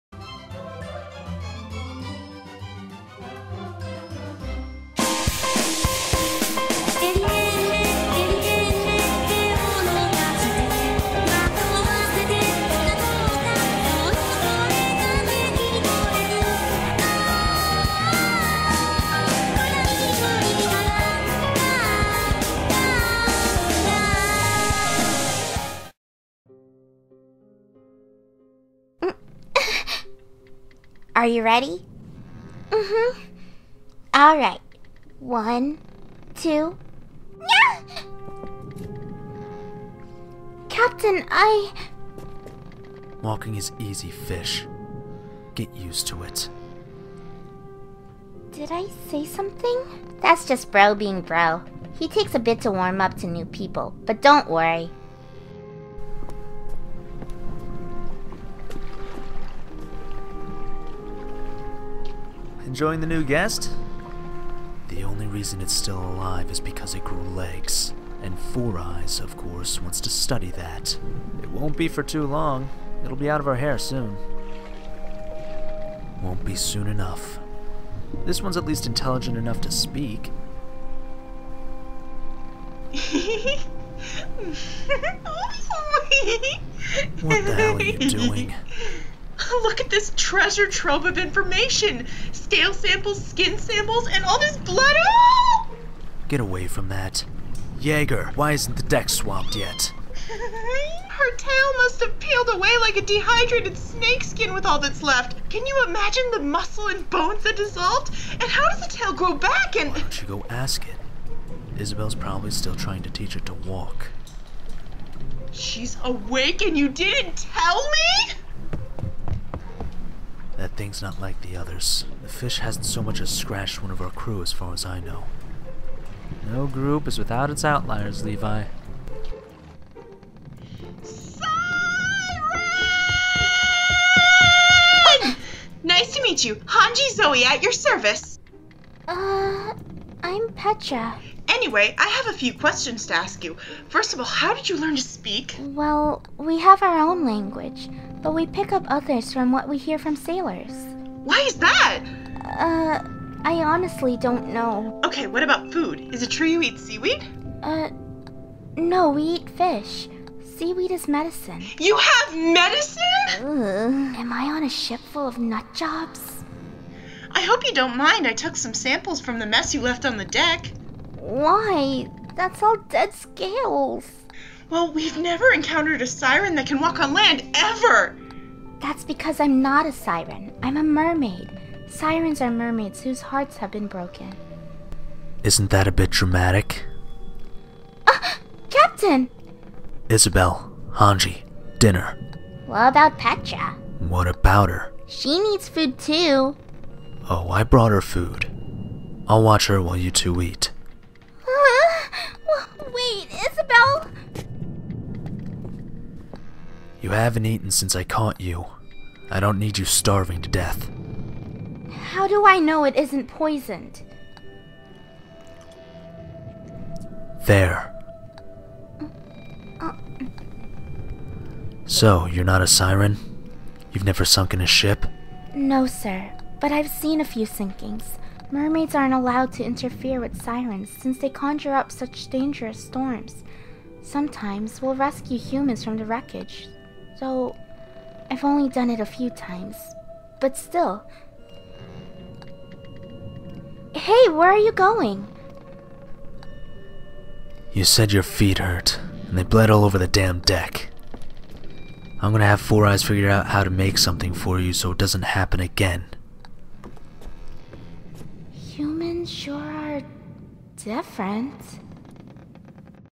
Serenade episode 3 (use headphones for the best quality)
episode 3: "what is a siren?"
(USE HEADPHONES FOR MAX QUALITY)
oh my God, I can't thank my voice actors, Princess Sakura Serenity and Isamu Mamoru enough for doing all the acting for this! I'm so happy how this came out (and the sound effects were a lot easier to do than I expected) and the motion will get better once I hook up my kinect to MMD and find some sort of lip synch program. For now, everything's by hand, so I hope you enjoy my weeb crap. **bows**

follow me on the bookface!!!

and on the twitter (**cough** which I never use **cough**): @SangoNanami